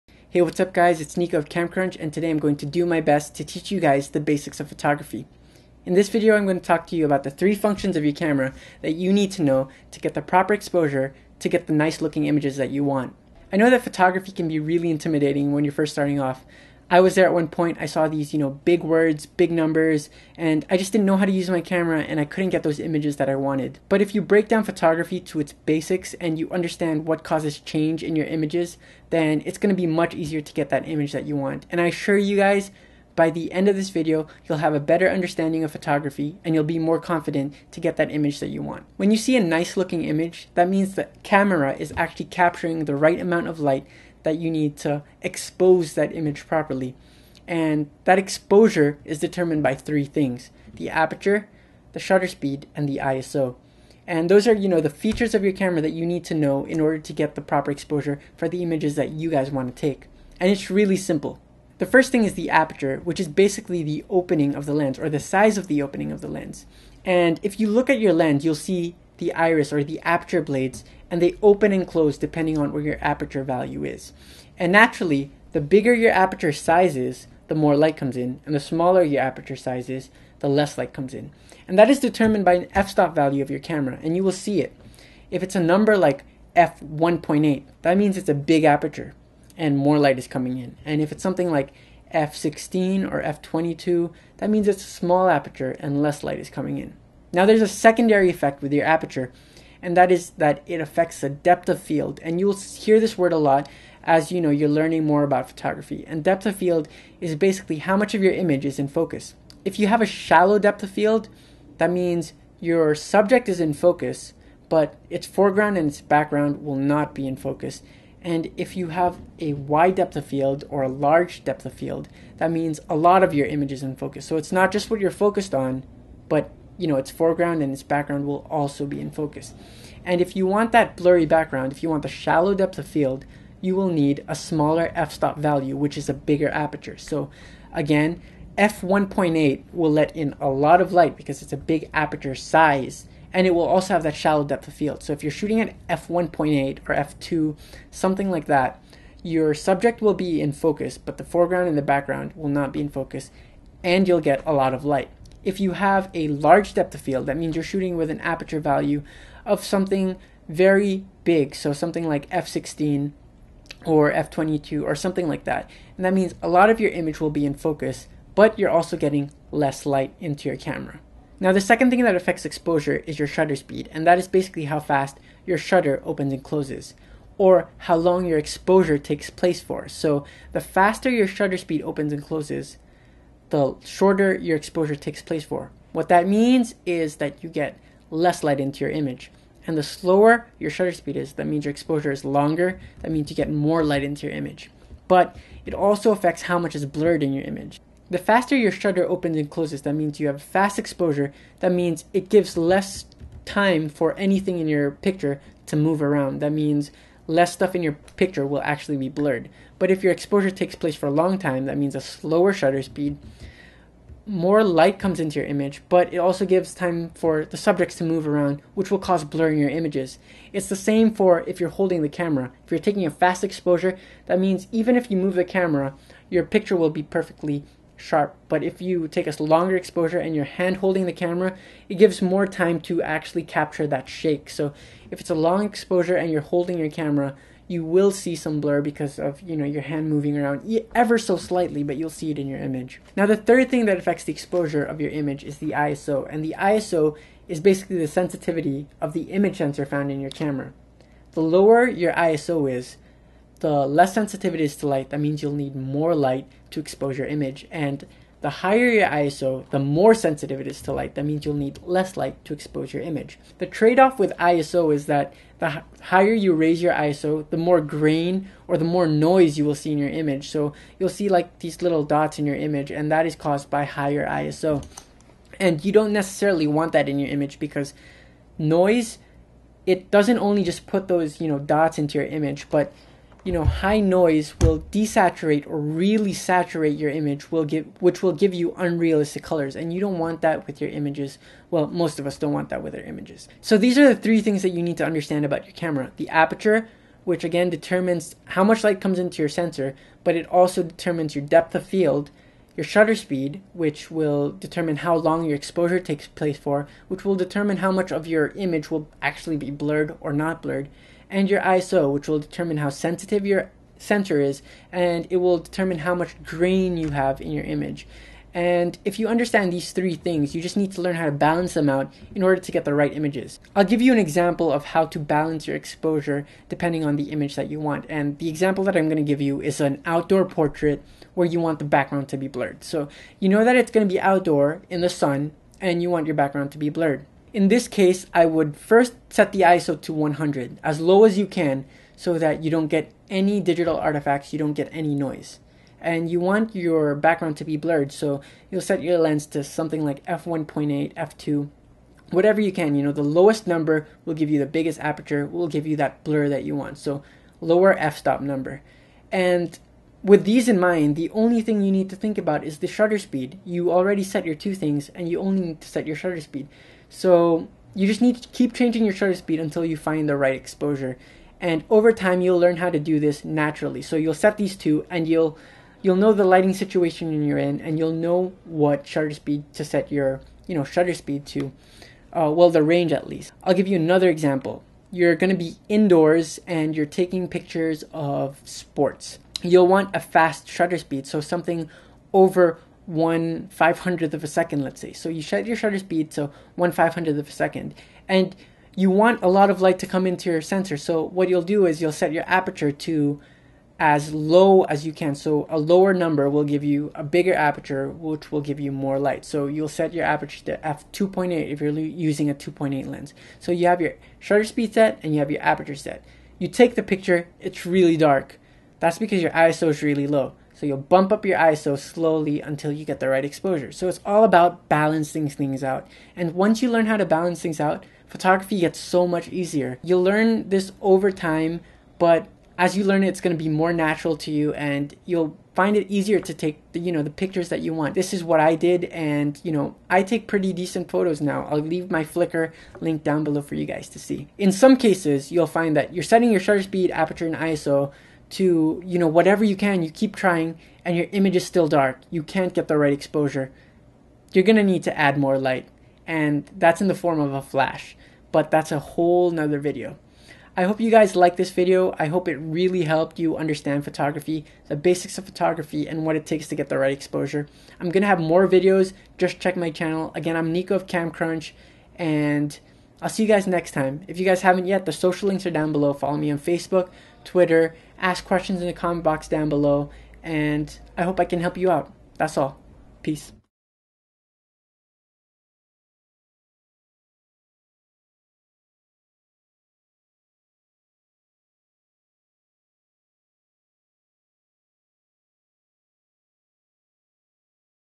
Photography Tutorial for Beginners: What is Aperture, Shutter Speed, ISO (DSLR Lesson) - CamCrunch
This is a photography tutorial for beginners. In this video, you will learn about the three things that affect the exposure of an image. After watching, you'll know exactly what the aperture, shutter speed and ISO do.

Video Summary

There three things that affect the exposure of an image: The APERTURE, SHUTTER SPEED and ISO. You need to find the perfect balance between these three elements to get the right look that you want, as well as the right exposure.

The aperture affects the amount of light that comes into the sensor and the depth of field. The depth of field will determine how much of the image is in focus.

The shutter speed will determine the length of the exposure. The longer the shutter speed, the more light comes in, and the shorter the shutter speed, the less light comes into the image. It also determines the amount of blur that appears in the photo. A longer exposure is more susceptible to blur and camera shake.

The ISO is the sensitivity of the sensor to light. Lower ISOs mean that the sensor is less sensitive and therefore going to need more light to expose an image. Higher ISO requires less light to expose and image, but adds noise the the image. Noise will saturate color, making them look unrealistic.

Getting the right exposure is all about balancing these three elements. It will take time, but if you learn the basics, it gets easier!

LINKS!